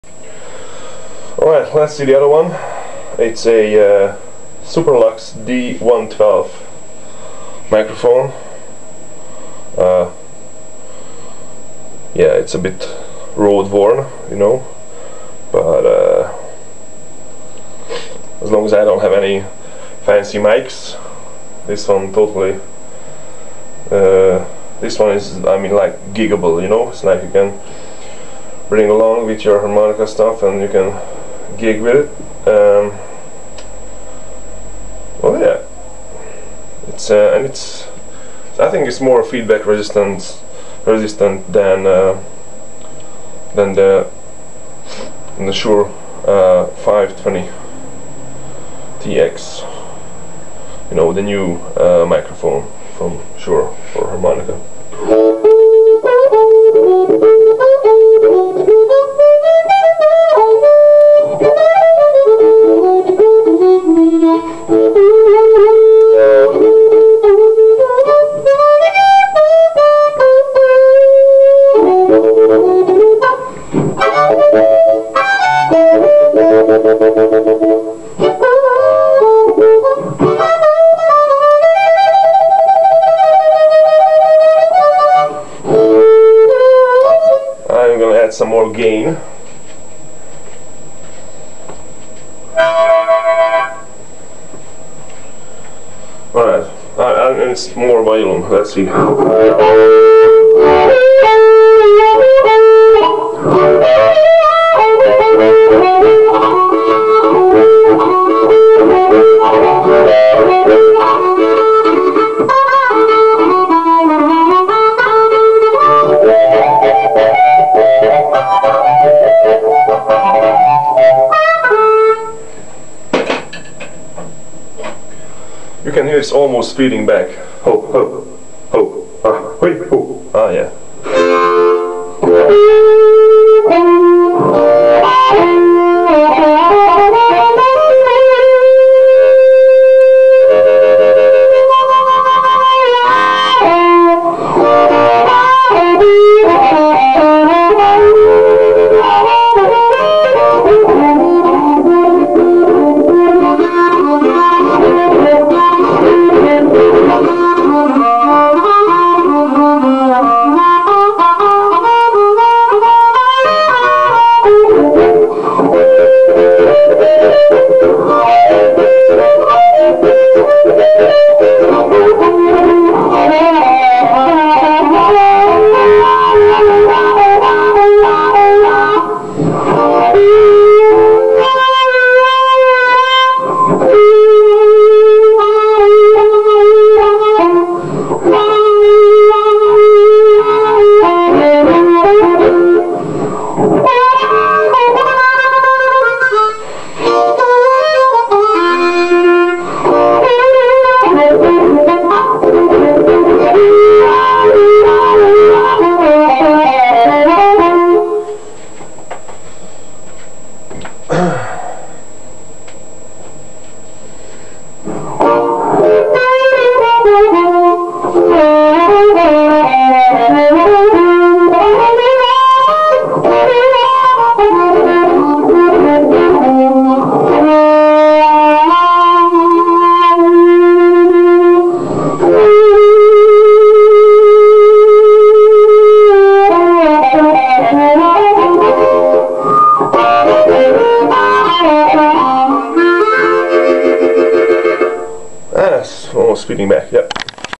Superlux D112 Harp Mic test with VHT Special 6 and Lone Wolf Harp Delay
Superlux d112 harp mic, unmodified. I think this mic sounds 95% the same as the Shure 520DX you can buy in most music stores. In my opinion, this mic is better than 520DX, because it sounds the same, more feedback resistant, weighs less, cord is stronger, and has 1/3 price of 5202DX. I am using a VHT Special 6 and a Lone Wolf Harp Delay. The LW delay is truly enhancing this mic, cutting back from feedback a bit, and fattening up the tone. This is the mic I usually gig with. I forgot to check the settings on the amp and the delay. Unfortunately, my crappy camera has a constant beep or hiss all the time, and its microphone is pretty weak also, and I'm still a beginner harp player. Whatever. Which mic sounds best? You tell!